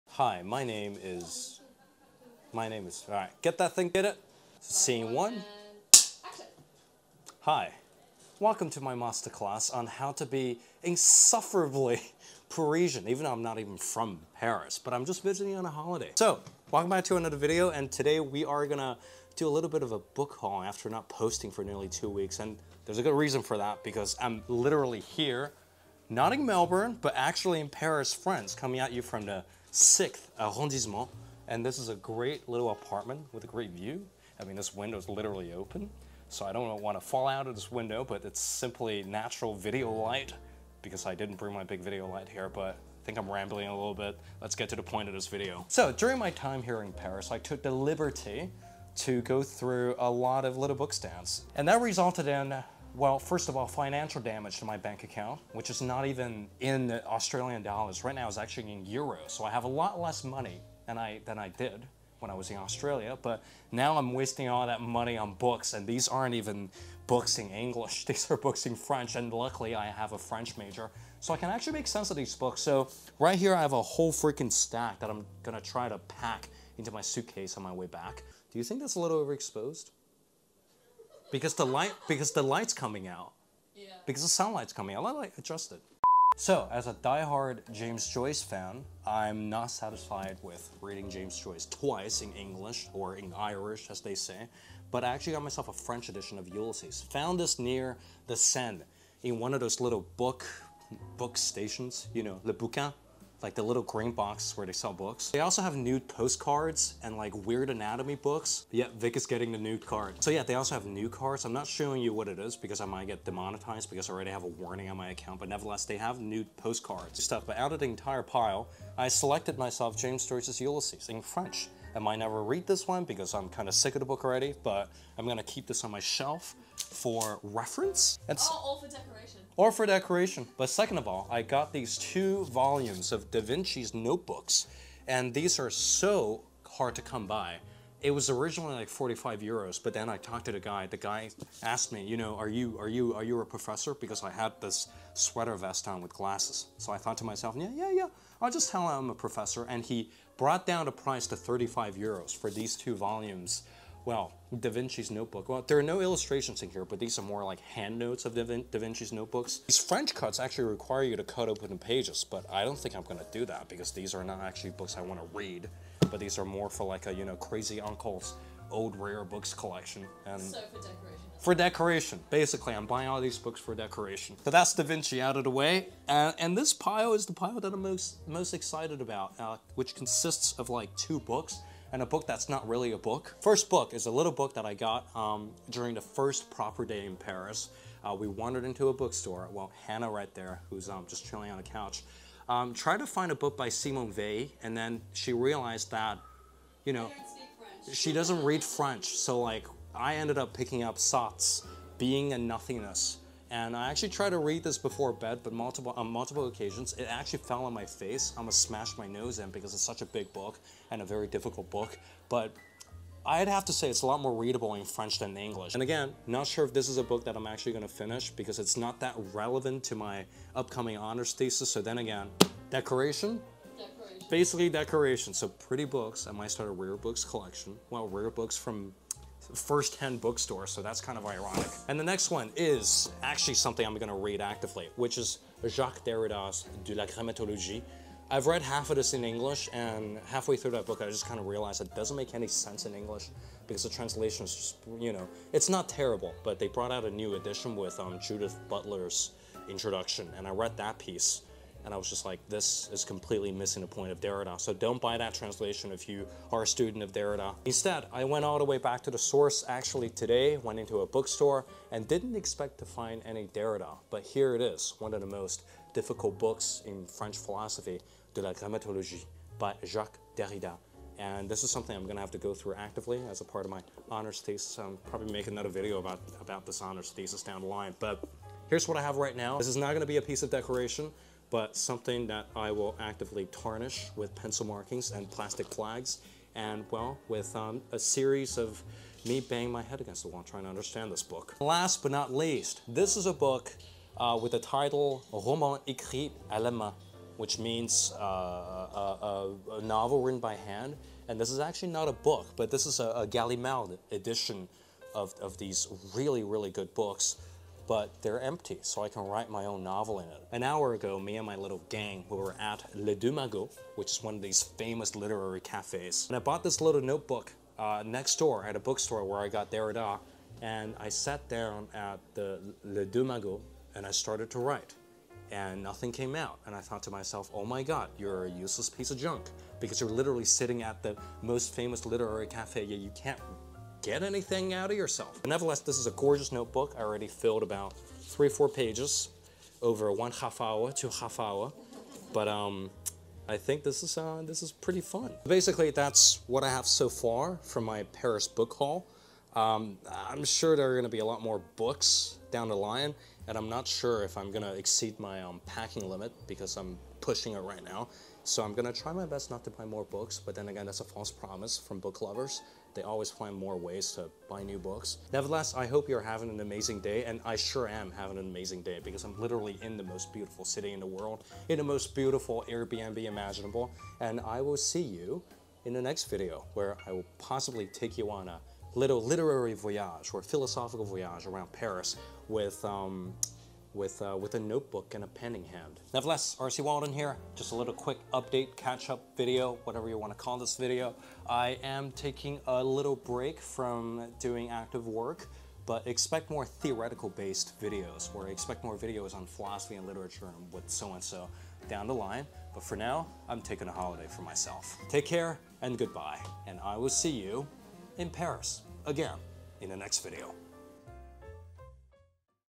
I Flew To Paris Just To Buy Books - A Book Haul
A catch-up video and some of the books I've been gathering during my trip to Paris.